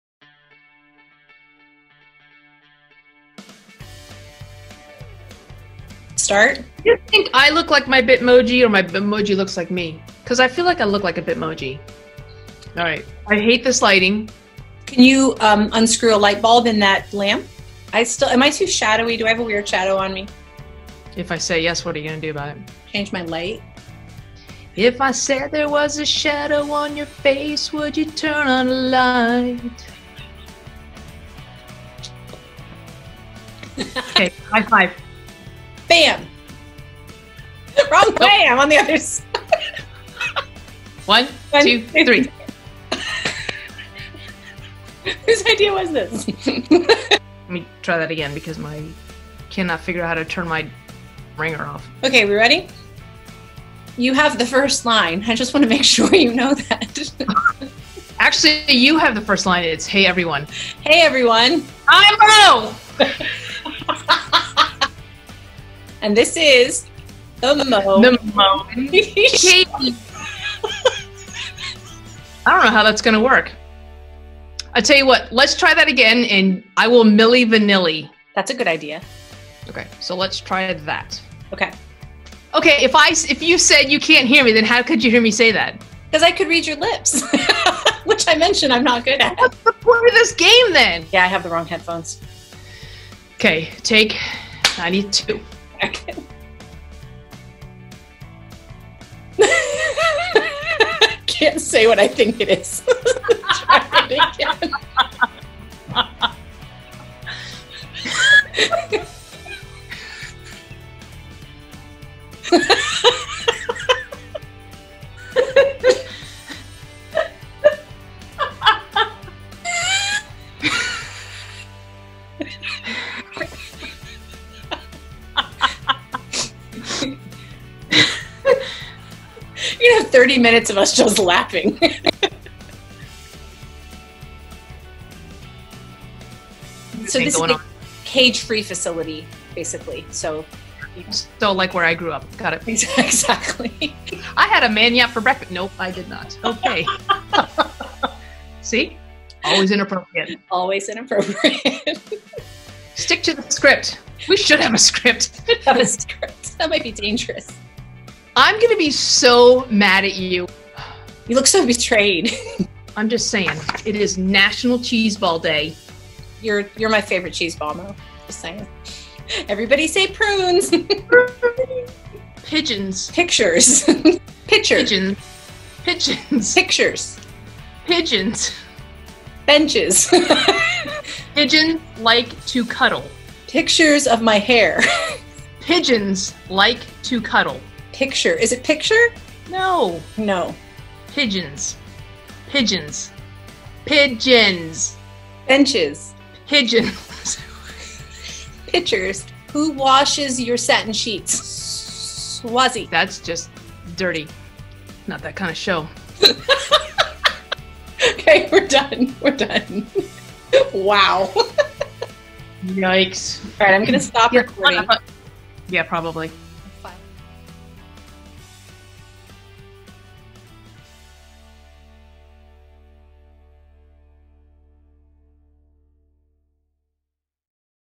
Mo & Katy Show Episode 4 Prep
A snippet of edits from the cutting room floor as we work out the kinks (time lag, zoom room high fives and lip reading among other things).